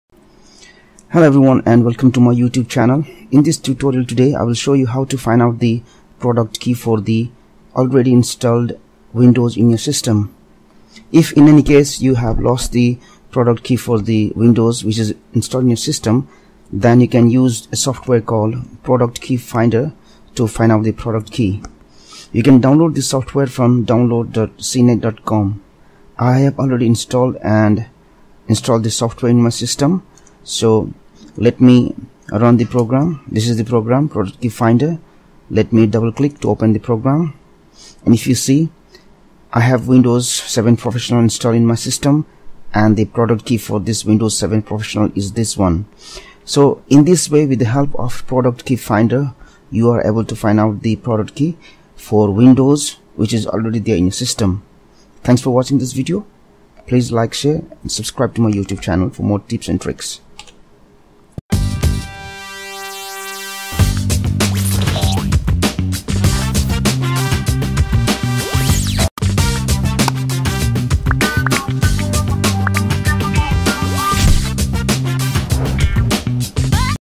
How to find product key for already installed Windows 7/ Windows 8 / Windows 8.1
If you have lost the product key for windows installed in your system then you can use Product key finder to find the key.